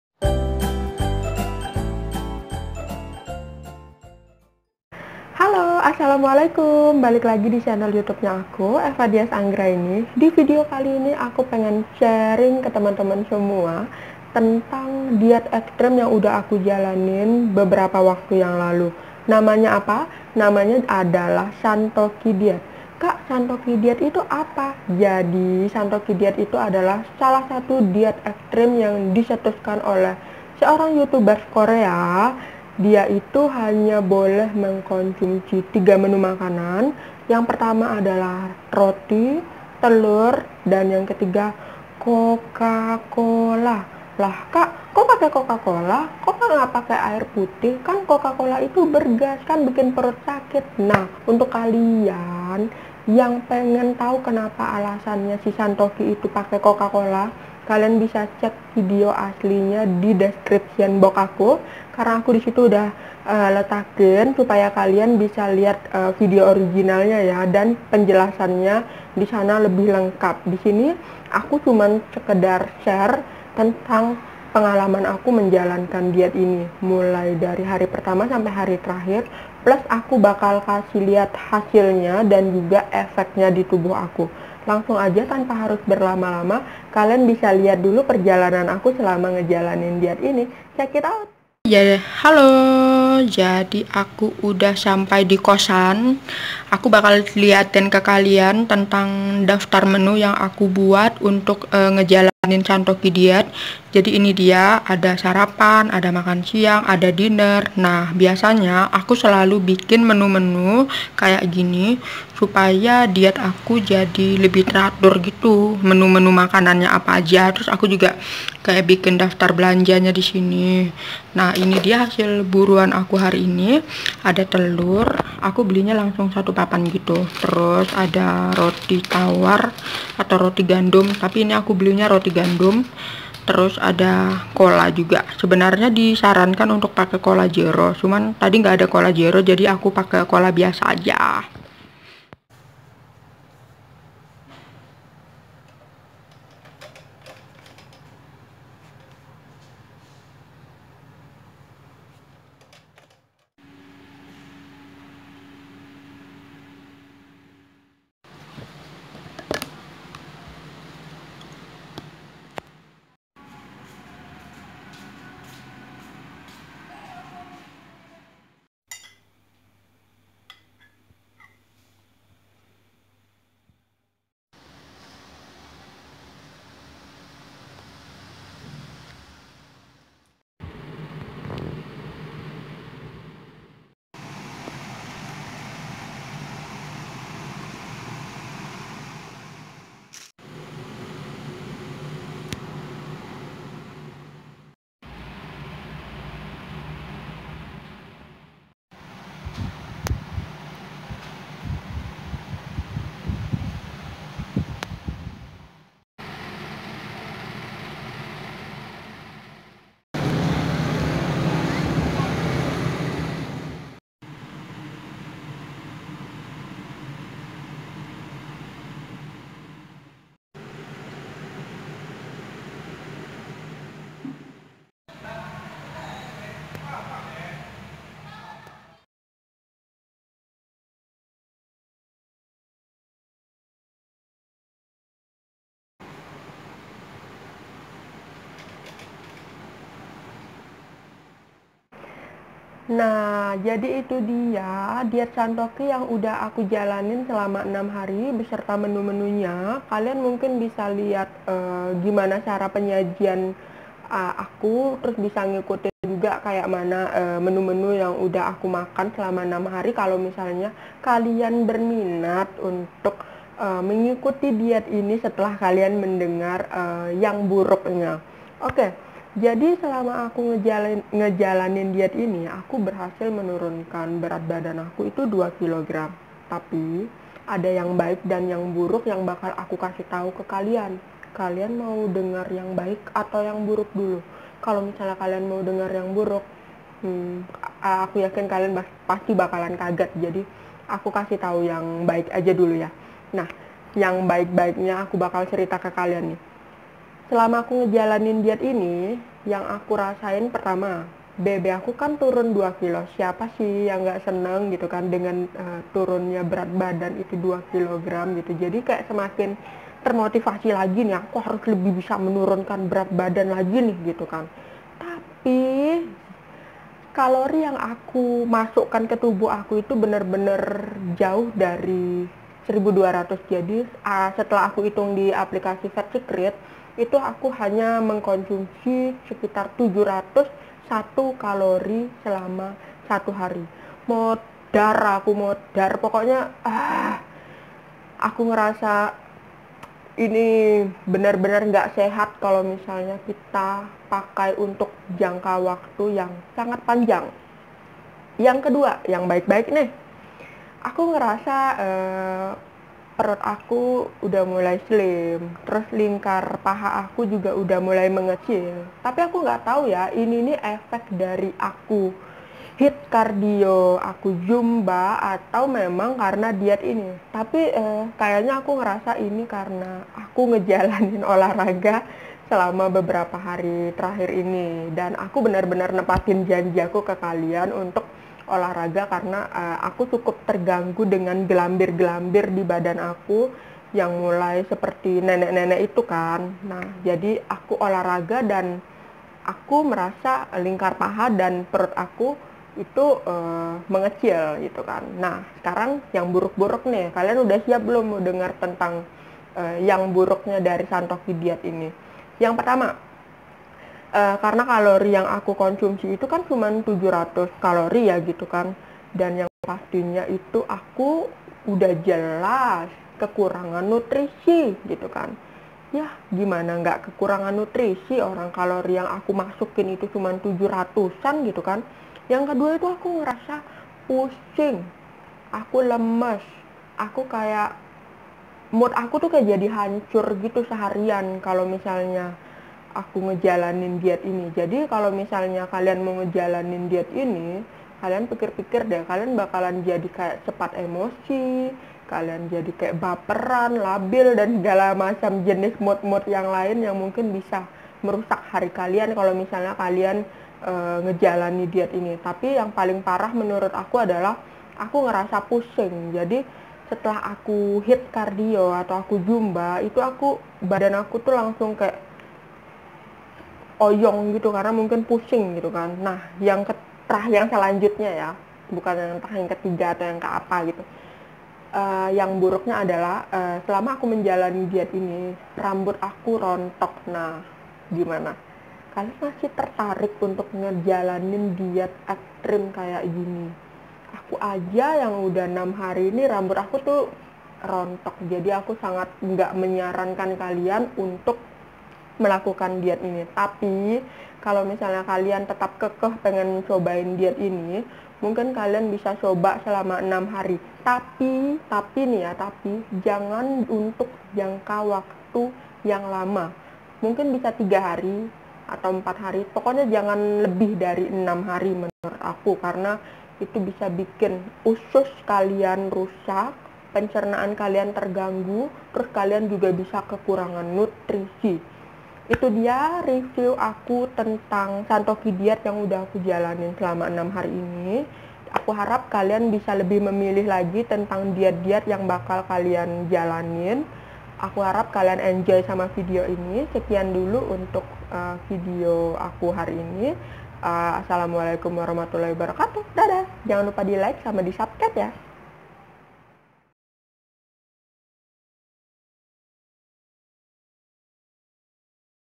#9 DietVlog : 6 hari turun 2 kg dengan Shantokki Diet 🤤
Halo... Assalamualaikum... liat video ini sampai akhir yaa biar kalian tau benefit dari diet ini apa. Aku harap kalian enjoy sama video ini yaa ❤